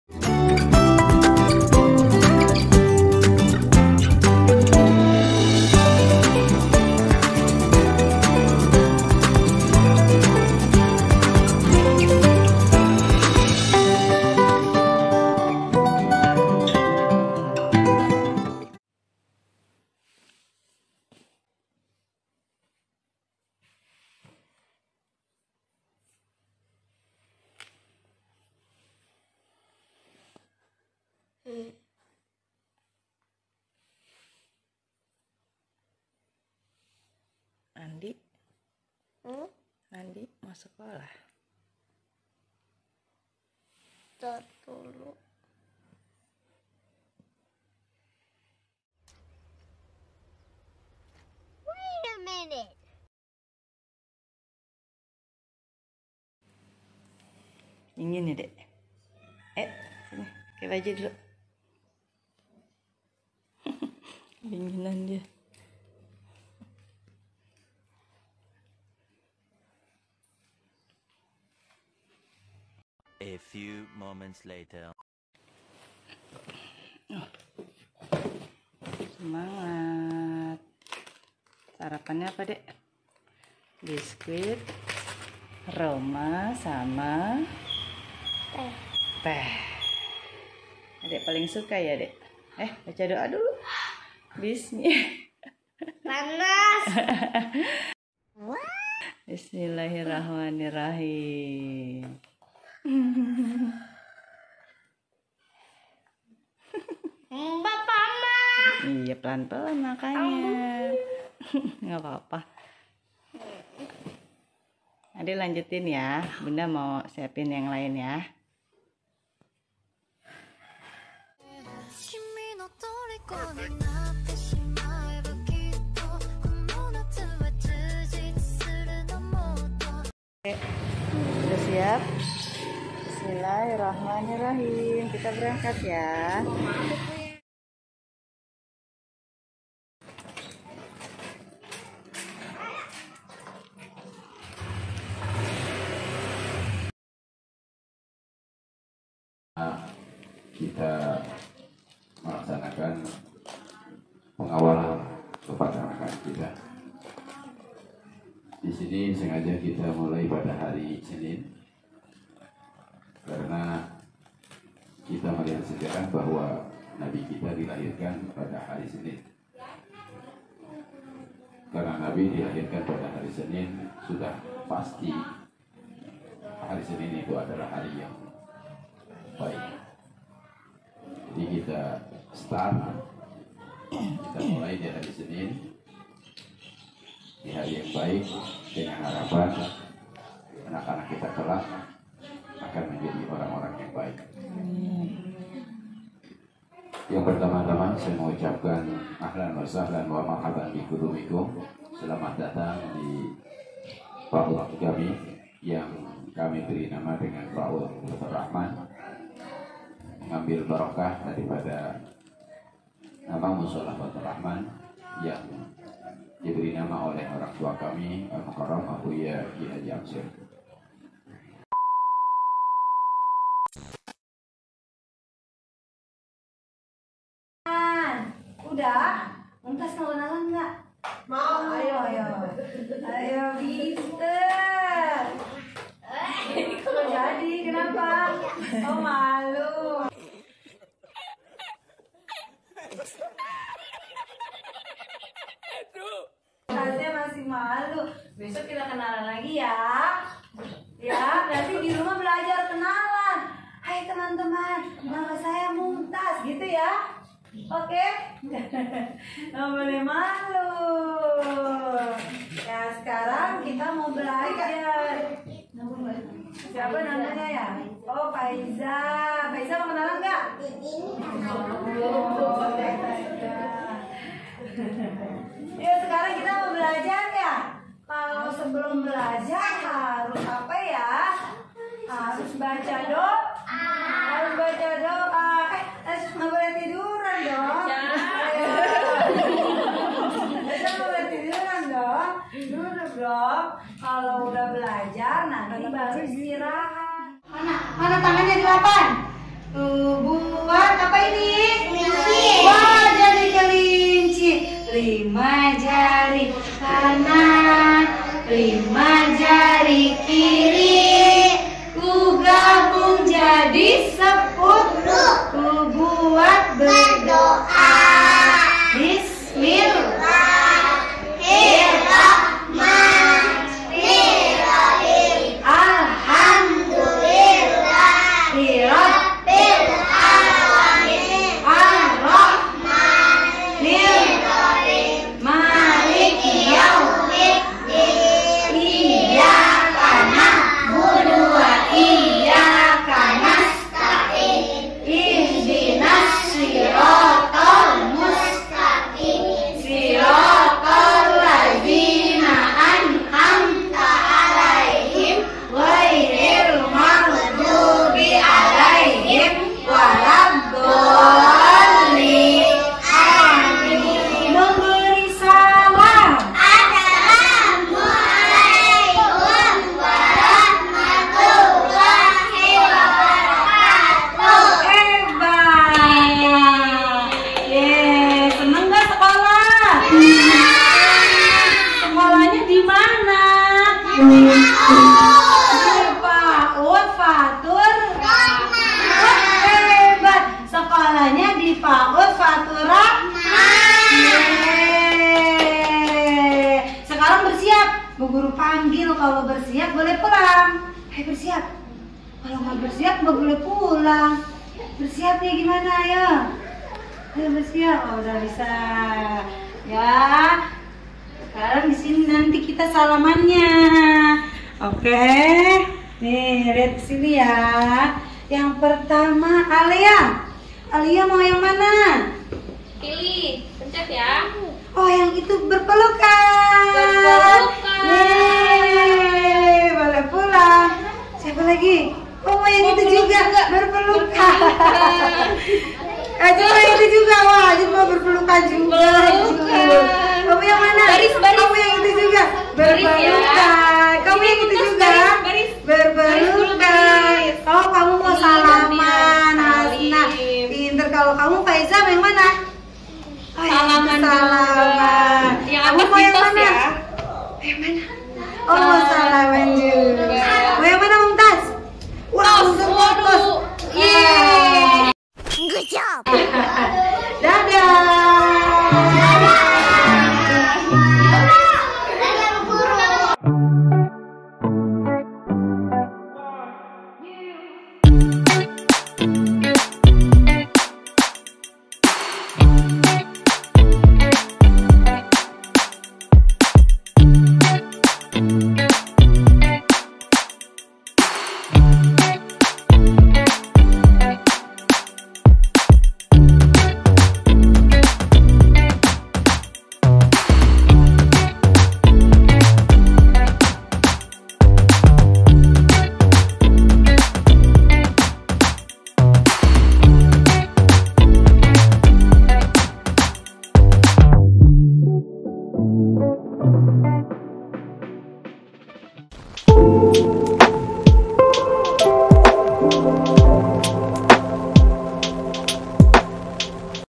FIRST DAY OF SCHOOL | PAUD FATHURRAHMAN
Assalamualaikum teman teman, hari ini adalah hari pertama ade mumtaz masuk sekolah di PAUD Fathurrahman. Di mulai dari morning activity yaitu sarapan. Setelah sarapan langsung cuss berangkat sekolah diantar bunda nya. Kegiatan belajar di PAUD Fathurrahman ini yaitu mulai pukul 08.00 WIB s/d 10.00 WIB. Adapun di hari pertama sekolah ini hanya pengenalan saja yaa teman teman.